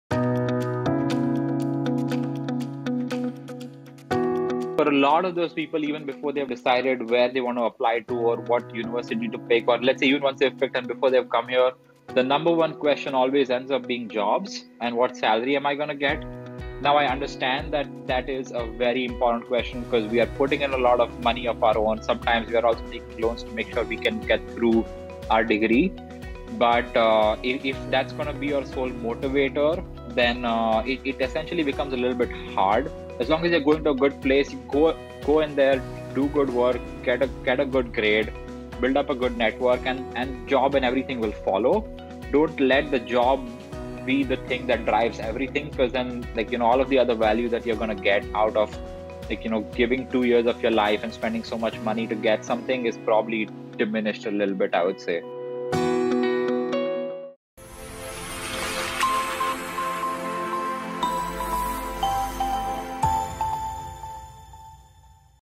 Important Advice from an Alumni of the University of Michigan
Meet Saaket Unadkat , a Collegepond Alumnus and graduate of University of Michigan. Saaket shares with you crucial advice from his/her experience at UMich.